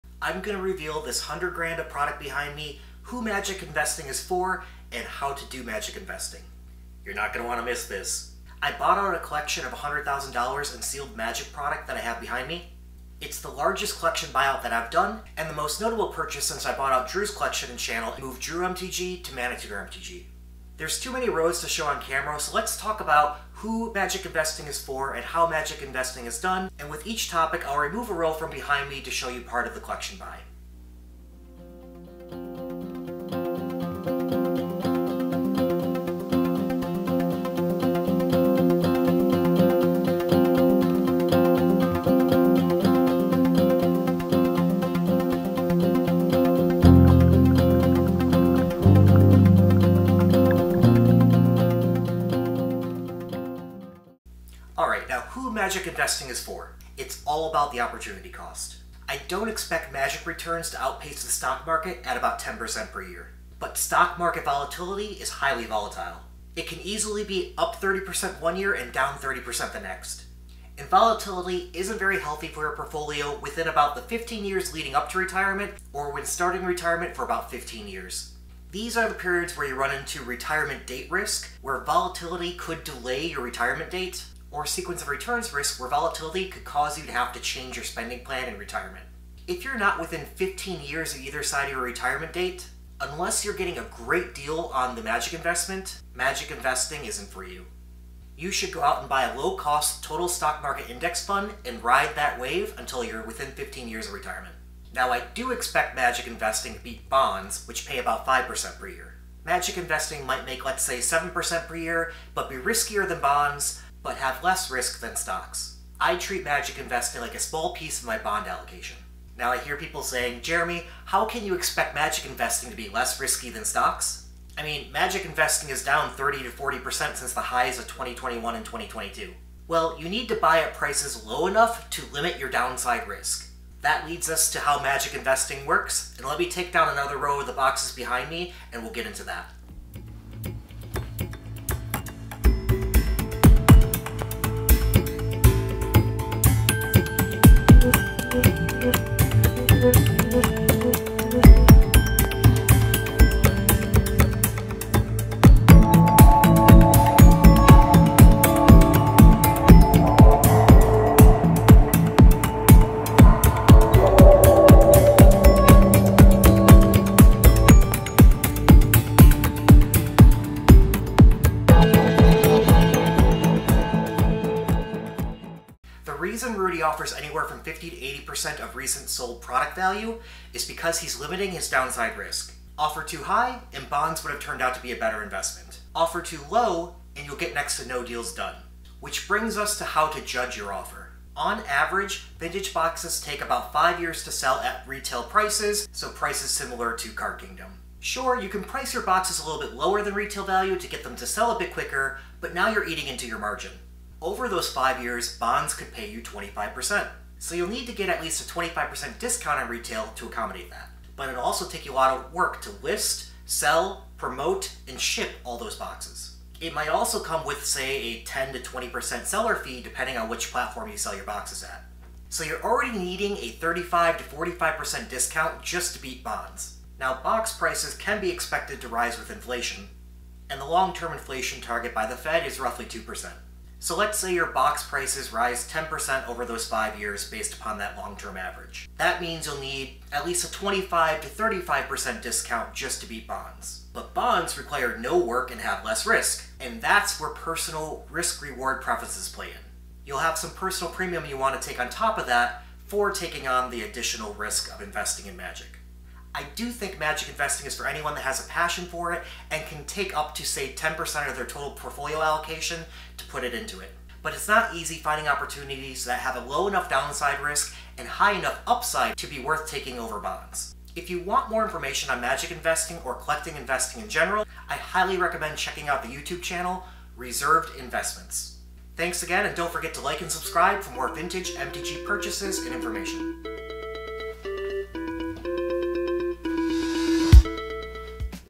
$100K MTG Collection Buyout
We'll review my $100K sealed MTG collection buyout, who Magic investing is for, and how Magic investing works.